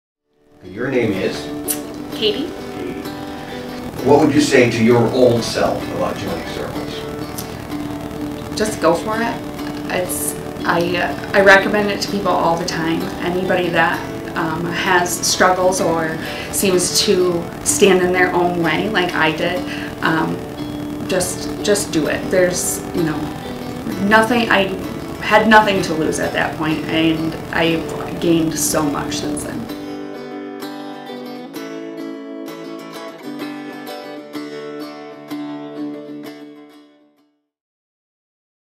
Kayte's story as a Circle Leader - part 8
Listen to what one of our Circle Leaders, Kayte K. would like to tell her old self about being in Circles Newaygo County.